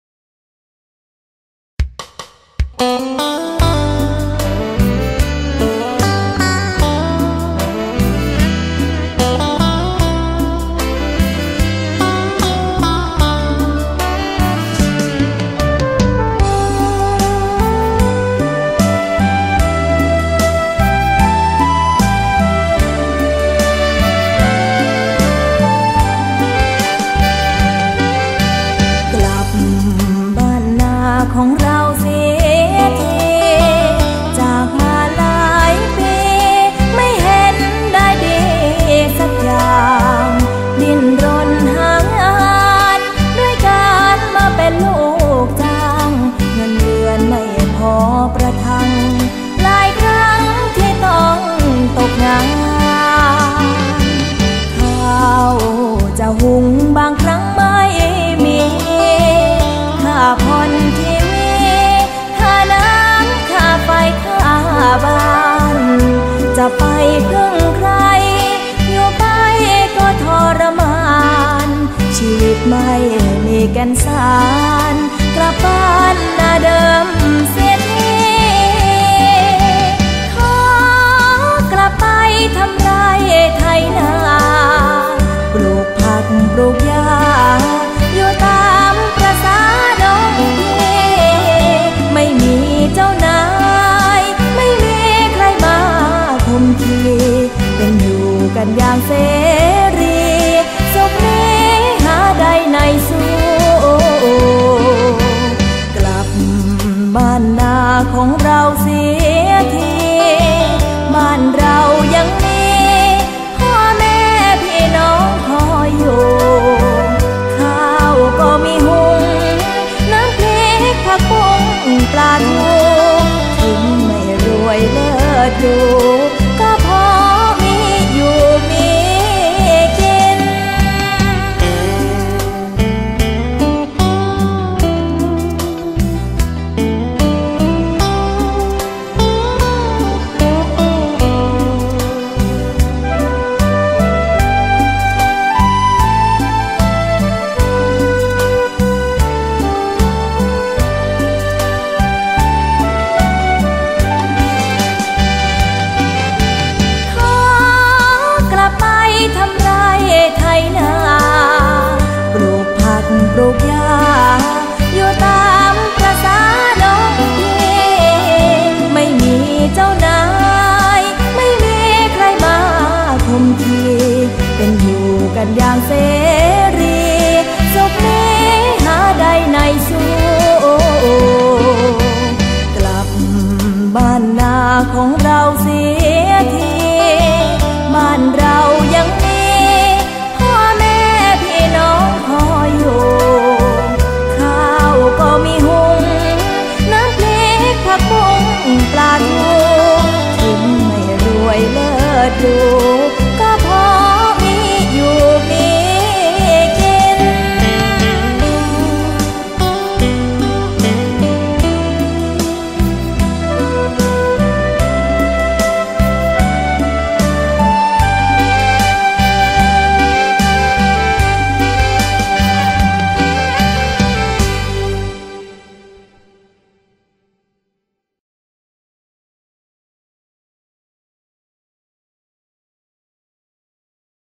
มาช่อมรถสาเล้ง
รถชาเล้งคันนี้มอบให้โดย บ.ไพทูรต์ น้ำแข็ง ต. ออม้ใหญ่ อ. สามพราน จ.นครปฐม